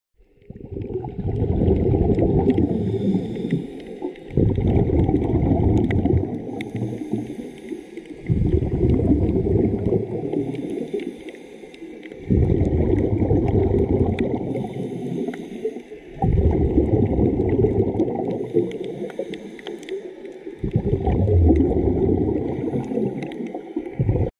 Pensacola Pass 8/3/22
Sergeant major fish at Pensacola Pass